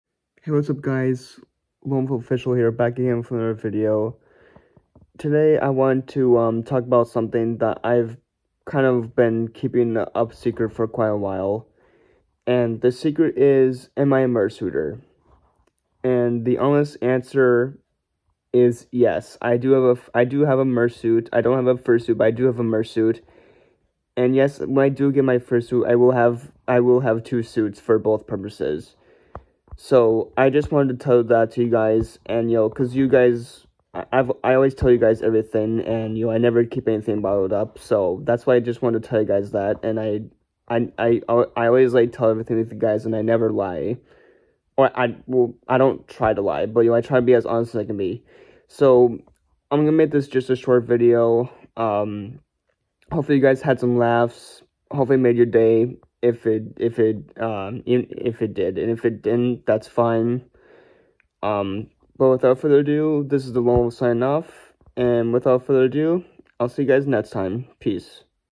Am I A Mursuiter?
I think its time I told y’all the truth, I do have one, and I don’t mind owning one and having it.